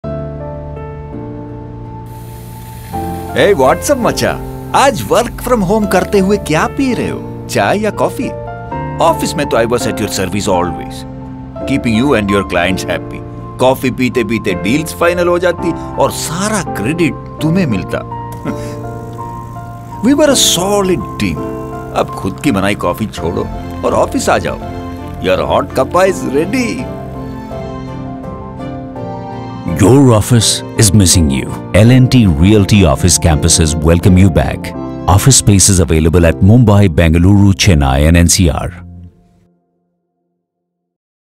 Your hot cuppa is ready | Office Calling | L&T Realty Commercial
Like how we are missing office, our office is also missing us. The stapler we used for so many years is looking forward to reunite with us. The coffee machine we started our day with is eagerly waiting to serve us a hot cuppa. The printer is lying unused and is hoping someone will soon give the command Ctrl+P. In this way, we have personified office objects who are pouring their hear out to all employees. Urging them to come back, now that things are fine. After all, who better than these office objects to invite us back to office? We all have special memories with them and share a special bond with them, so their appeal will not go unheard.